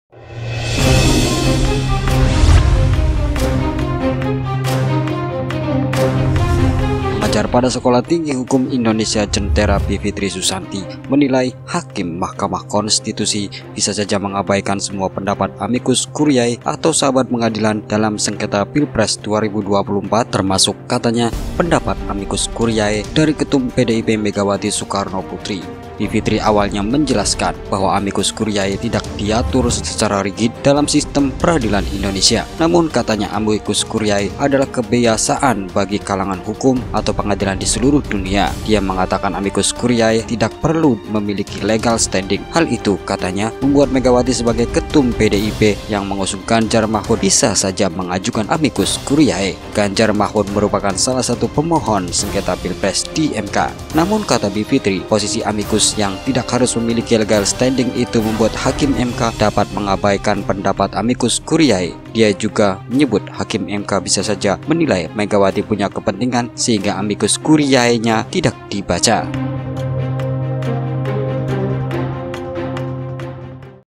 SARAT KEPENTINGAN DAN TAK PUNYA LEGAL STANDING.. HAKIM MK BISA ABAIKAN AMICUS CURIAE MEGAWATI
Pengajar pada Sekolah Tinggi Hukum (STH) Indonesia Jentera, Bivitri Susanti, menilai hakim Mahkamah Konstitusi (MK) bisa saja mengabaikan semua pendapat amicus curiae atau sahabat pengadilan dalam sengketa Pilpres 2024. Termasuk, katanya, pendapat amicus curiae dari Ketum PDIP Megawati Soekarnoputri.
Bivitri awalnya menjelaskan bahwa amicus curiae tidak diatur secara rigid dalam sistem peradilan Indonesia. Namun, katanya, amicus curiae adalah kebiasaan bagi kalangan hukum atau pengadilan di seluruh dunia.

Dia mengatakan amicus curiae tidak perlu memiliki legal standing. Hal itu, katanya, membuat Megawati sebagai Ketum PDIP yang mengusung Ganjar-Mahfud, bisa saja mengajukan amicus curiae. Ganjar-Mahfud merupakan salah satu pemohon sengketa Pilpres di MK.//detik//